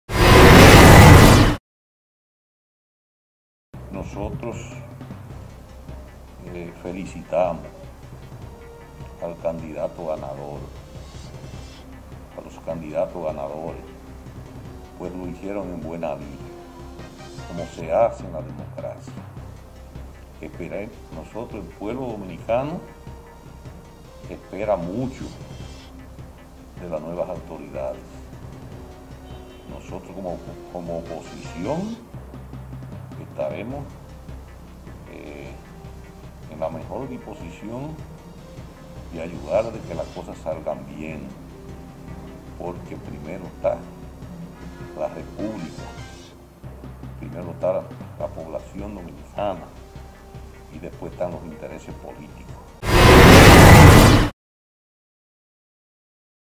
Gobernador SFM felicita a nuevas autoridades
Luis Núñez Pantaleón, gobernador de la provincia Duarte resaltó la valencia y alto nivel de civismo de parte de los dominicanos al darse cita y efectuar su voto, pese al escenario que se vive ante la pandemia, misma que mantuvo a la nación en Tres meses de cuarentena.

“Bueno primero hay que felicitar a los dominicanos por el grado de civismo, que exhibió, pues fuero masivamente, como siempre lo ha hecho, quienes distorsionan las cosas, somos a veces los dirigentes” expresó el gobernante.

Núñez Manifestó su felicitación a los nuevos ganadores, alegando que lo hicieron en buena lid, alegando que así es que se hace democracia.

El Gobernador provincia Duarte, resaltó “Nosotros los dominicanos esperamos mucho de las nuevas autoridades, nosotros como oposición estaremos en la mejor disposición de ayudarle a que las cosas salgan bien, porque primero está la república, primero está la población y luego los intereses políticos.

El gobernante dijo que espera que en este nuevo gobierno se siga profundizando en pro del desarrollo del país y que se pueda mantener la estabilidad económica.